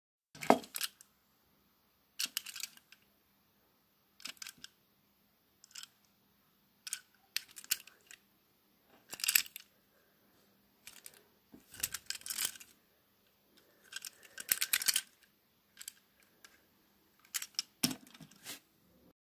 Thomas & Friends Adventures Nia
A Showcase of Nia from the Thomas & Friends Adventures line.